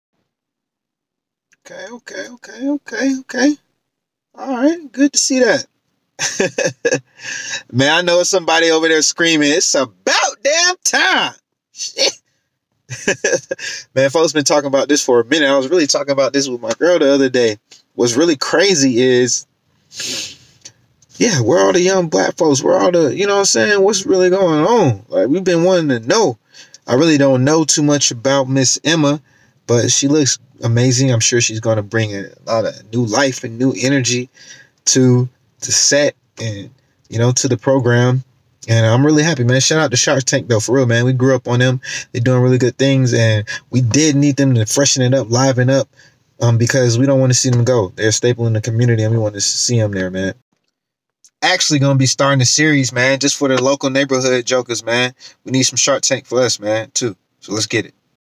Shark Tank Adds First Black Woman - Mastermind Behind Kim Kardashian Apparel
Congratulations Emma. Can't wait to tune in.